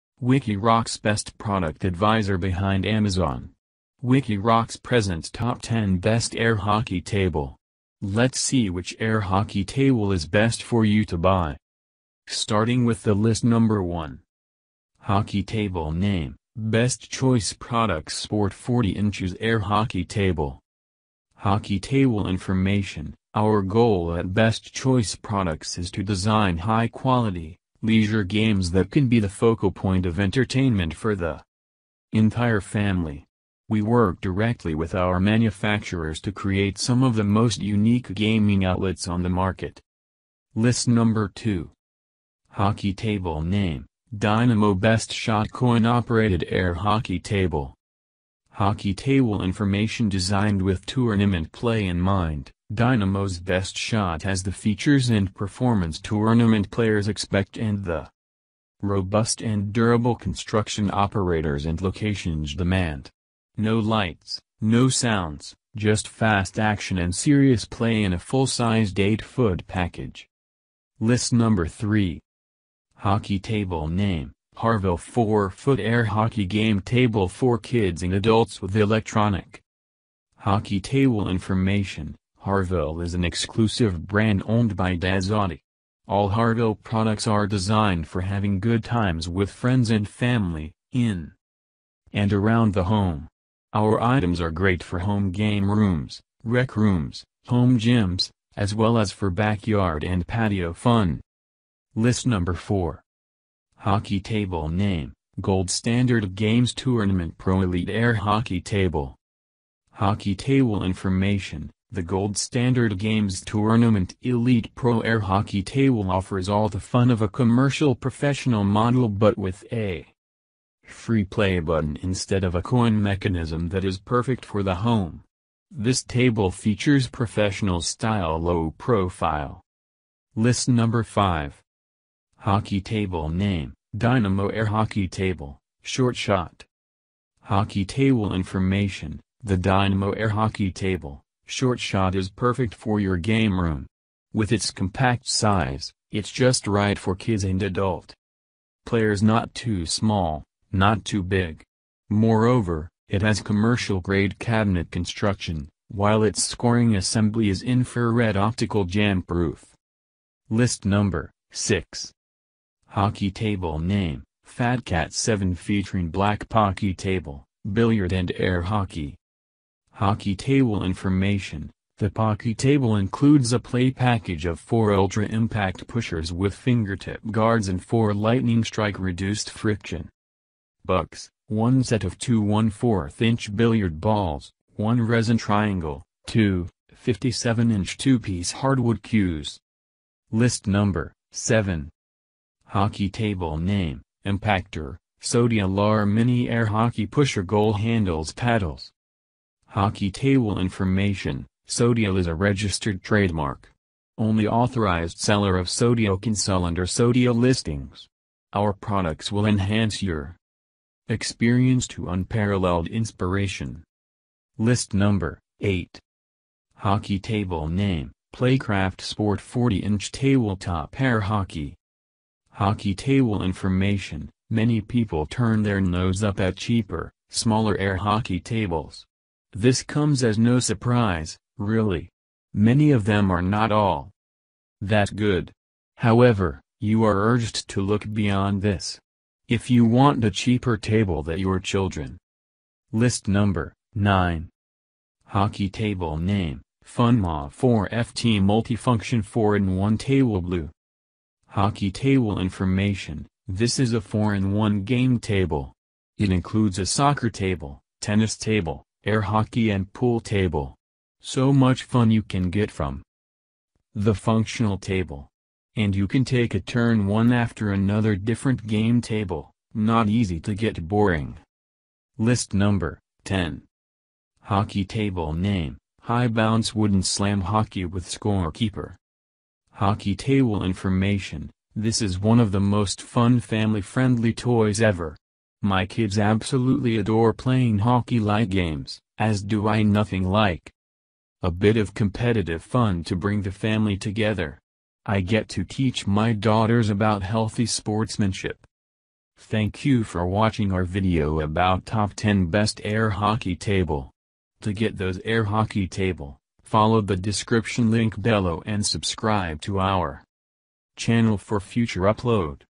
10 Best air hockey table 2020
Number 1
Number 2
Number 3
Number 4
Number 5
Number 6
Number 7:
Number 8
Number 9
Number 10:

This video is about 10 Best air hockey table you will get best idea about air hockey table.

Hockey table name: Best Choice Products Sport 40" Air Hockey Table.

Hockey table  information: Our goal at Best Choice Products is to design high quality, leisure games that can be the focal point of entertainment for the entire family. We work directly with our manufacturers to create some of the most unique gaming outlets on the market.

List number 2,

Hockey table  name: Dynamo Best Shot Coin Operated Air Hockey Table.

Hockey table  information:Designed with Tournament play in mind, Dynamo’s BEST SHOT has the features and performance Tournament players expect and the robust and durable construction Operators and Locations demand. No lights, no sounds, just fast action and serious play in a full-sized 8-foot package.

List number 3,

Hockey table  name: Harvil 4 Foot Air Hockey Game Table for Kids and Adults with Electronic.

Hockey table  information: Harvil is an exclusive brand owned by Dazadi. All Harvil products are designed for having good times with friends and family, in and around the home. Our items are great for home game rooms, rec rooms, home gyms, as well as for backyard and patio fun.

List number 4,

Hockey table  name: Gold Standard Games Tournament Pro Elite Air Hockey Table.

Hockey table  information: The Gold Standard Games Tournament Elite Pro Air Hockey Table offers all the fun of a commercial Professional model but with a free-play button instead of a coin mechanism that is perfect for the home. This table features professional-style low profile.

List number 5,

Hockey table  name: Dynamo Air Hockey Table - Short Shot.

Hockey table  information: The Dynamo Air Hockey Table - Short Shot is perfect for your game room! With its compact size, its just right for kids and adult players--not too small, not too big. Moreover, it has commercial-grade cabinet construction, while its scoring assembly is infrared optical jam-proof.

List number,6

Hockey table  name: Fat Cat 7 ft. Black Pockey Table - Billiard & Air Hockey.

Hockey table  information: The Pockey Table includes a play package of 4 ultra-impact pushers with fingertip guards and 4 lightning-strike reduced friction pucks, 1 set of 2-1/4 inch billiard balls, 1 resin triangle, 2, 57 inch two-piece hardwood cues.

List number,7

Hockey table  name: impactor - SODIAL(R)Mini Air Hockey Pusher Goal Handles Paddles.

Hockey table  information: SODIAL is a registered trademark. ONLY Authorized seller of SODIAL can sell under SODIAL listings. Our products will enhance your experience to unparalleled inspiration.

List number,8

Hockey table  name: Playcraft Sport 40-Inch Table Top Air Hockey.

Hockey table  information: Many people turn their nose up at cheaper, smaller air hockey tables. This comes as no surprise, really. Many of them are not all that good. However, you are urged to look beyond this. If you want a cheaper table that your children.

List number,9

Hockey table  name: Funmall 4FT Multi-function 4 in 1 Table Blue.

Hockey table  information: This is a 4 in 1 game table. It includes a soccer table, tennis table, air hockey and pool table. So much fun you can get from the functional table. And you can take a turn one after another different game table, not easy to get boring.

List number,10

Hockey table  name: High Bounce Wooden Slam Hockey With Score Keeper.

Hockey table  information: This is one of the most fun family friendly toys ever! My kids absolutely adore playing hockey like games, as do I. Nothing like a bit of competitive fun to bring the family together! I get to teach my daughters about healthy sportsmanship.